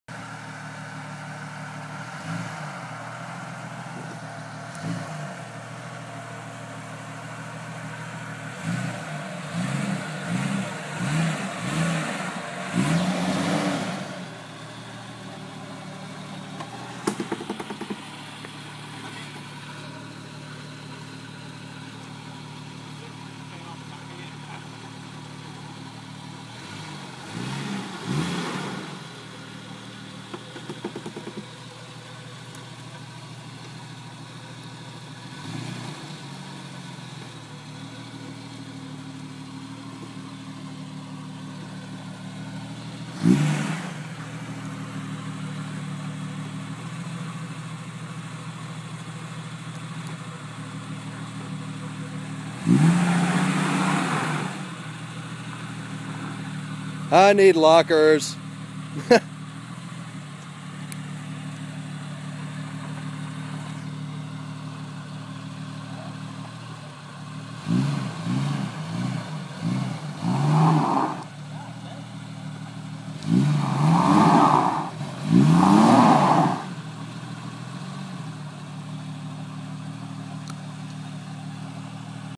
Takin the Jeep Offroad for the first time, Lawnmower Driving
Lawnmower and I doin a little trail ridin.